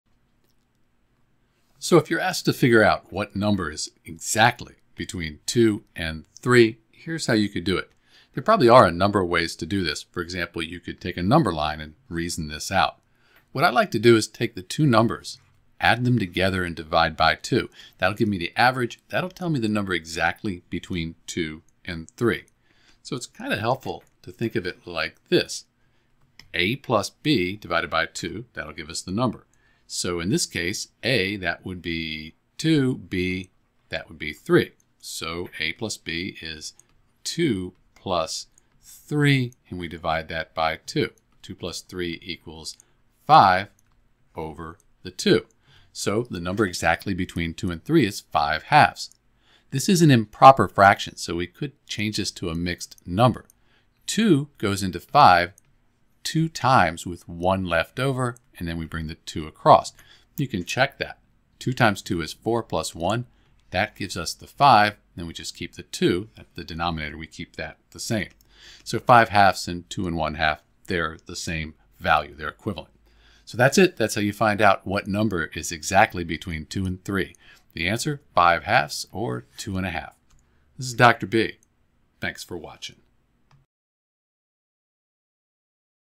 What number is exactly between 2 and 3?
To find the number exactly between 2 and 3 using the formula (a + b) / 2, follow these steps:

Identify the given numbers: In this case, you have a = 2 and b = 3.

Apply the formula: Plug the values of a and b into the formula (a + b) / 2.

We can write 5/2 as a mixed number, 2 ½.  As a decimal this is 2.5.

So, the number exactly between 2 and 3 is 2.5.

This method is particularly useful when dealing with very large or very small numbers because it avoids potential errors that could arise from visual estimation on a number line.

In this case, even if you were to reason it out on a number line, you would come to the same conclusion: the midpoint between 2 and 3 is indeed 2.5.